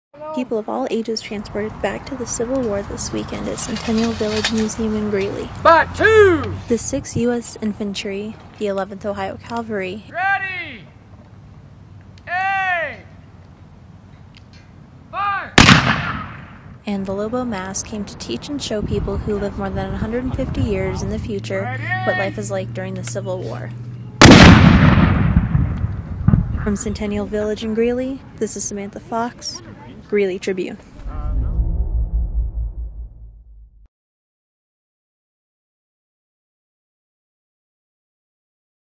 The Civil War came to Greeley Saturday and Sunday!
By: Samantha Fox
Published on: September 20, 2015